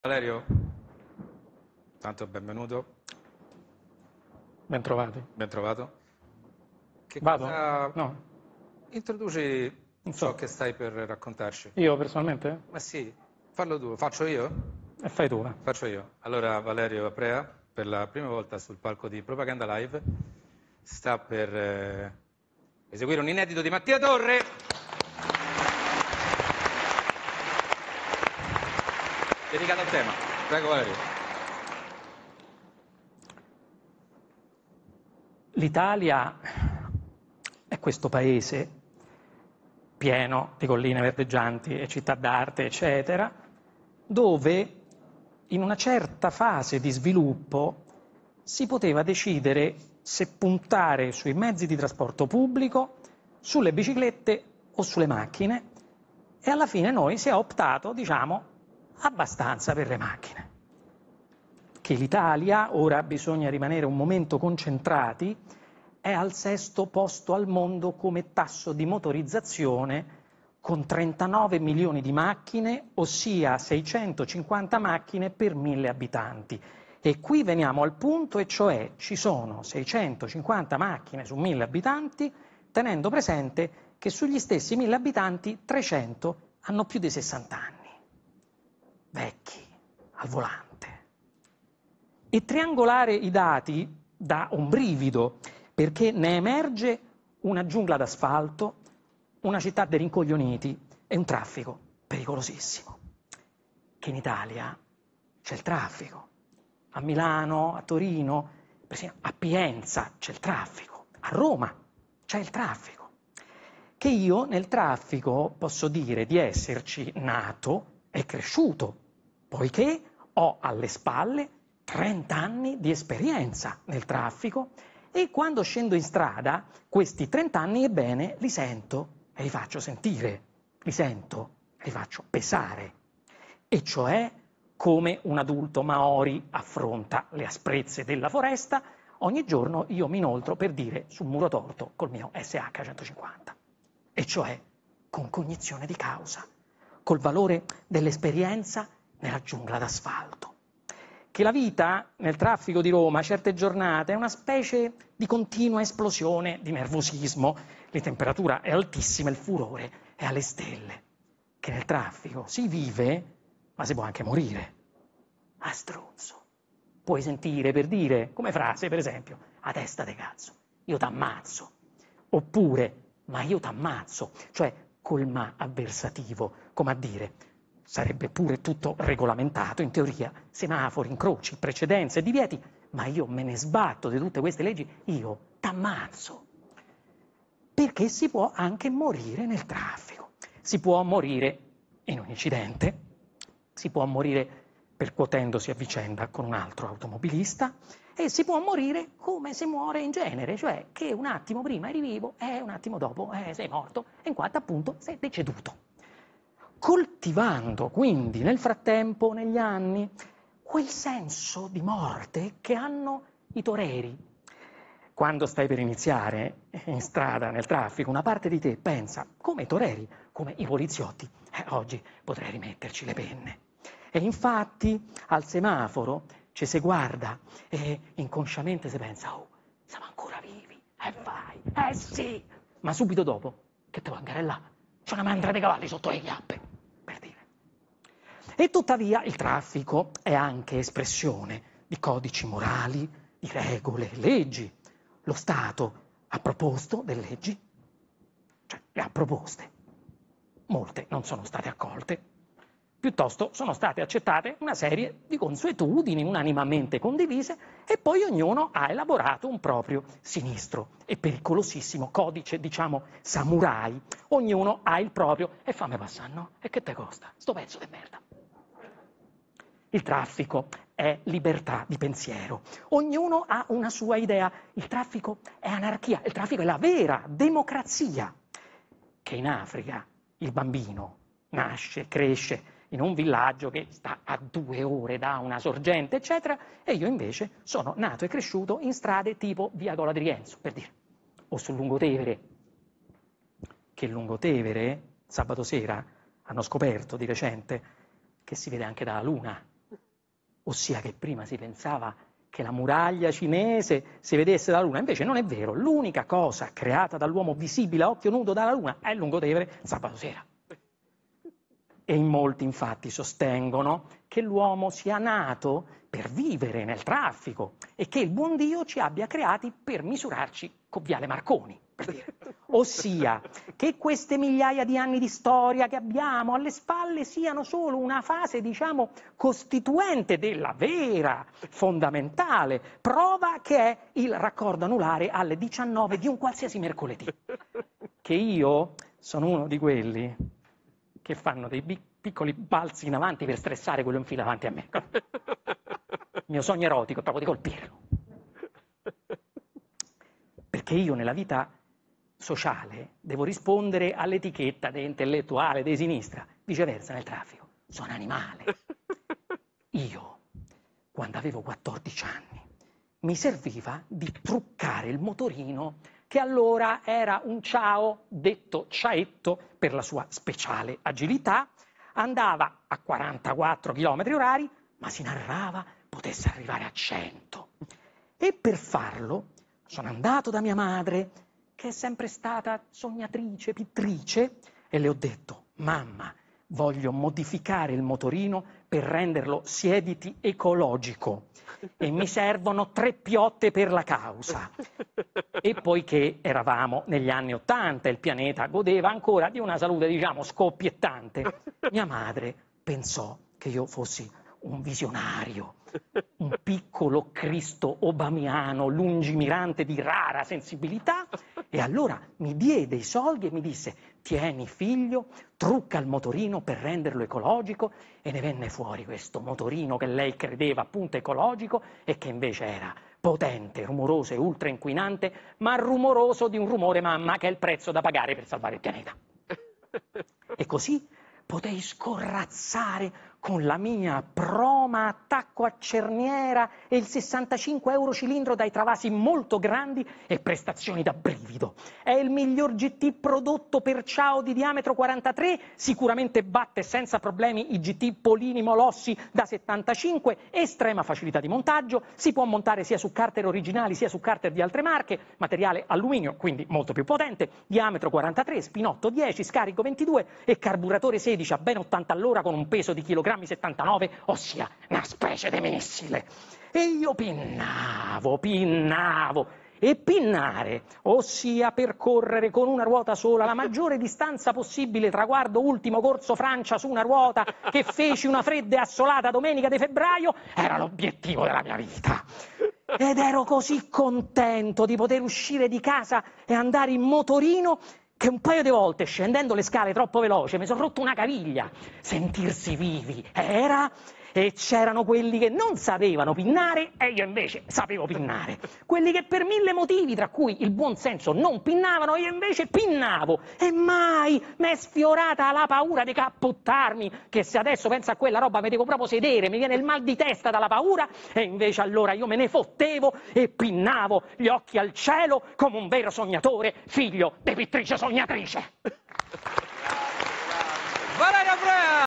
"Traffic": Valerio Aprea legge il monologo di Mattia Torre
"Traffic", Valerio Aprea legge il monologo inedito di Mattia Torre